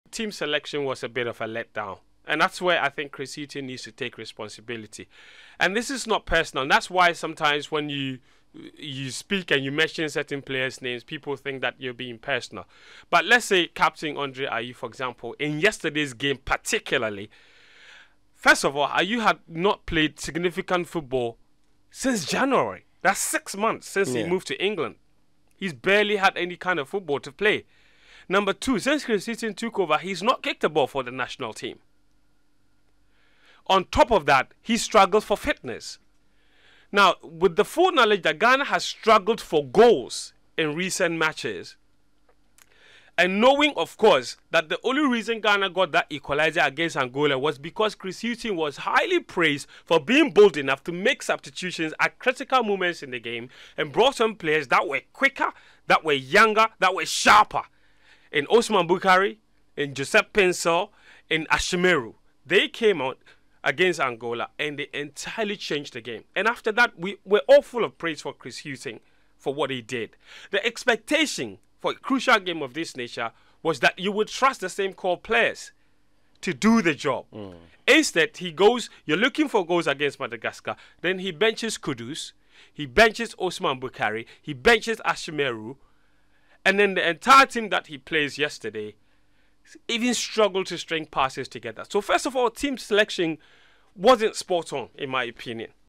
Team selection was a let-down and Chris Hughton needs to take responsibility for that
Black Stars: Team selection was a let-down and Chris Hughton needs to take responsibility for that.

- Fentuo Tahiru Fentuo